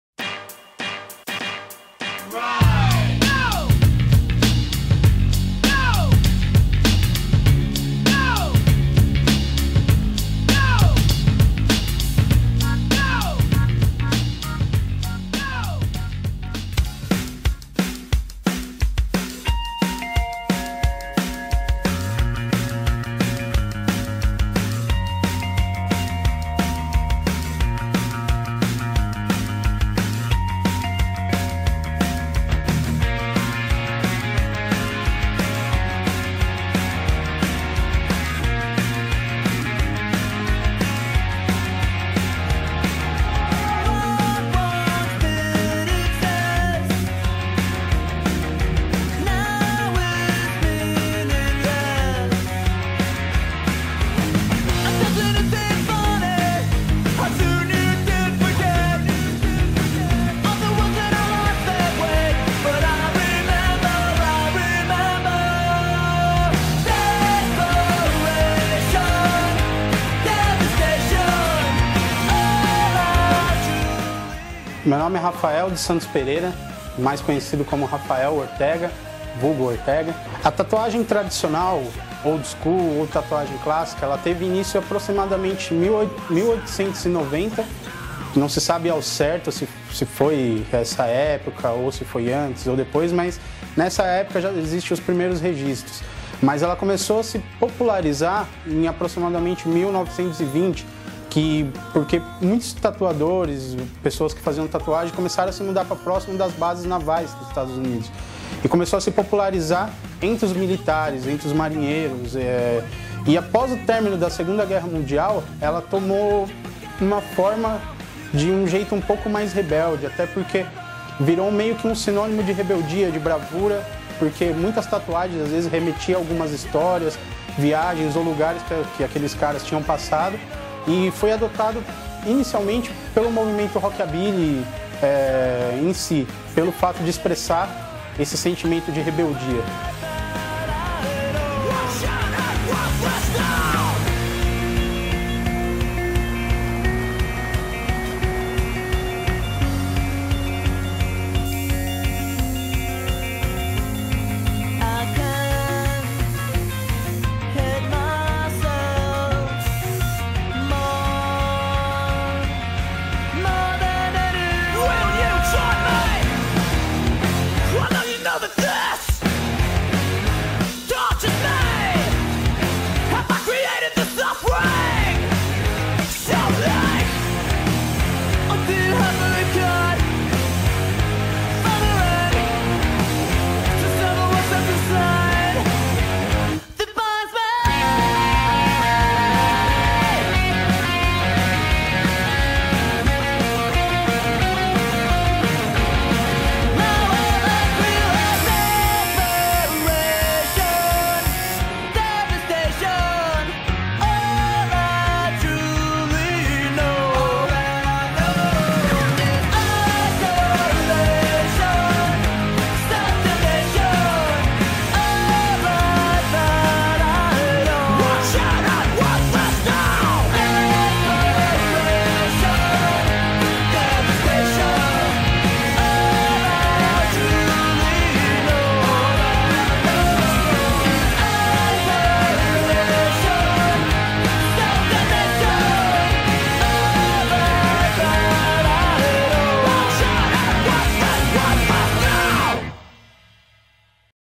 Rafael Ortega (traditional Tattoo)
Video que mostra um pouco do trabalho do tatuador Rafael Ortega, e fala um pouco sobre a tatuagem tradicional oldschool. Obrigado a todos que visualizaram o video, que apoiam nosso trabalho e a todos clientes e amigos que nos movitvam cada vez mais nessa caminhada.